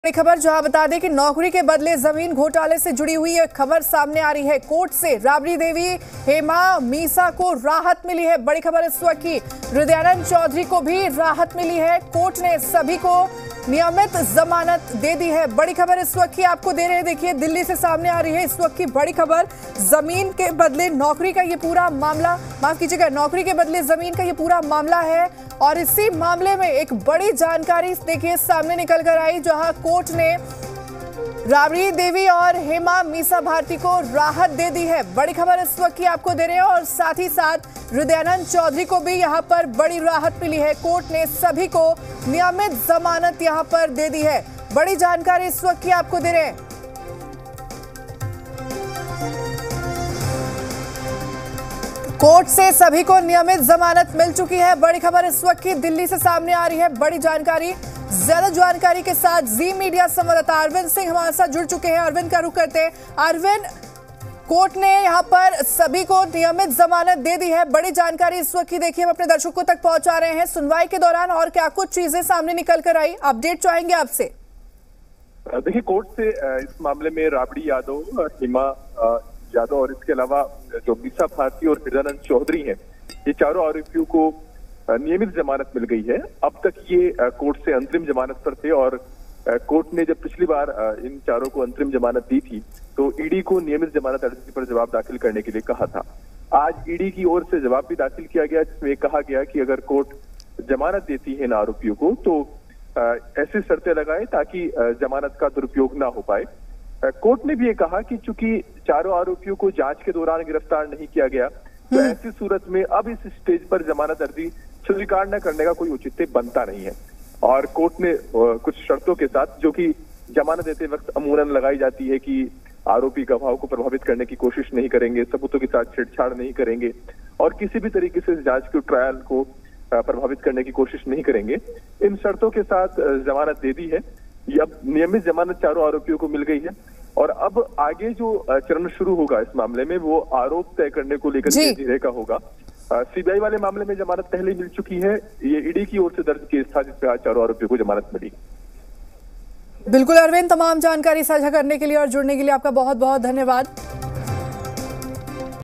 Land For Job Case: Court से Rabri Devi, Hema Yadav और Misa Bharti को राहत | Lalu Yadav | Bihar
Land For Job Case: नौकरी के बदले जमीन घोटाले के मामले में कोर्ट से Rabri Devi, Hema Yadav और Misa Bharti को राहत...Court से सभी को मिली नियमित जमानत...देखिए पूरी ख़बर...